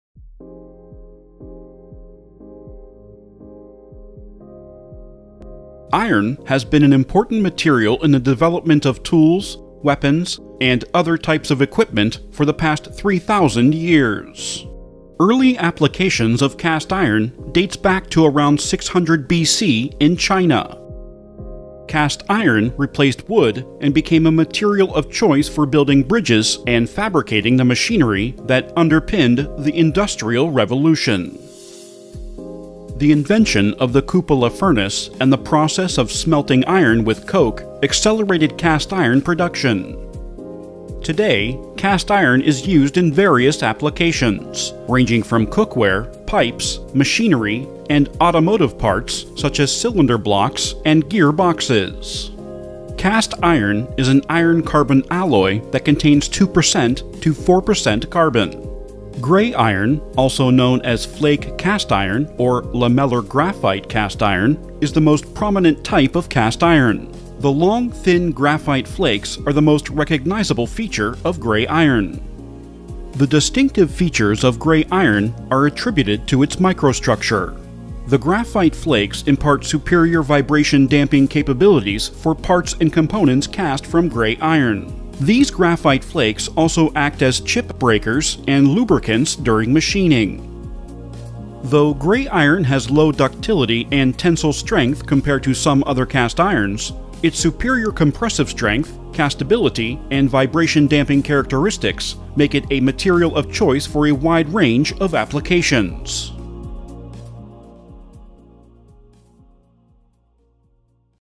What is Gray Iron? ll THORS Lightning Learning
What is Gray Iron?

Gray Iron Fundamentals introduces the learner to the world of cast irons, including the properties, processes, and terminology associated with gray iron production.